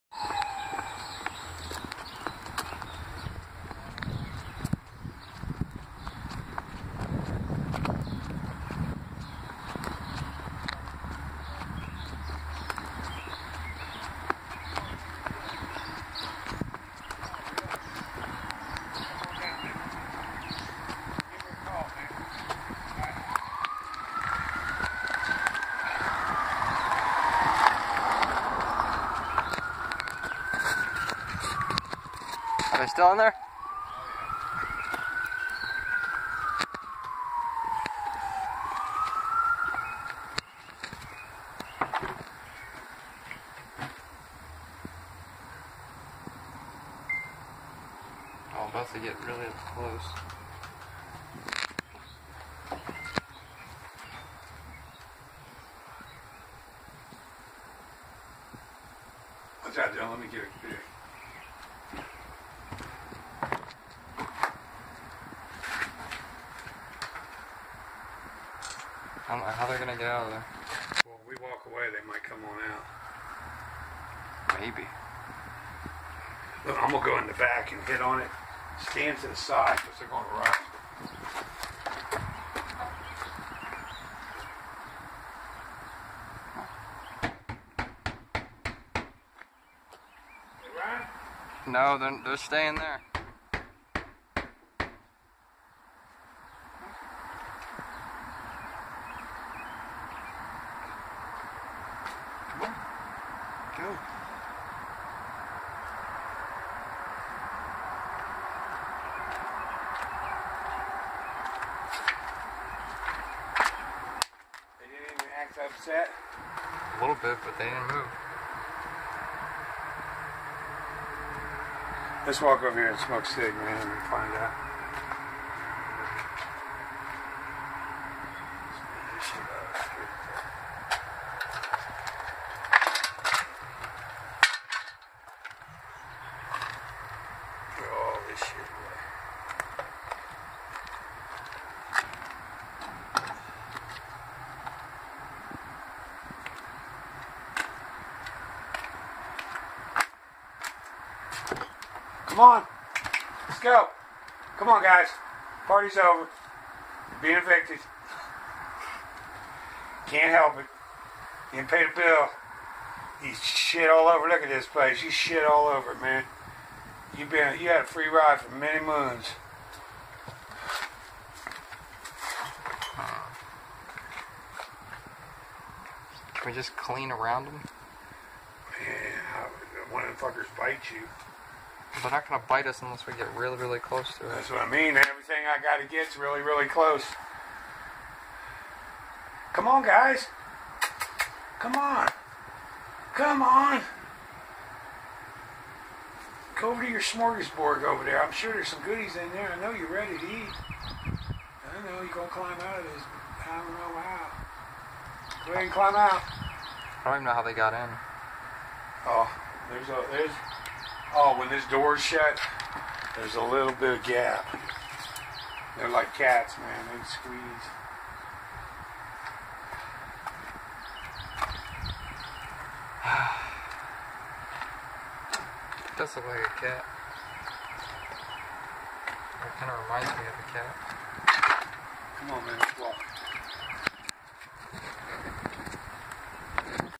Raccoons hiding in nasty shed...
Found some raccoons in a shed that smelled like hell.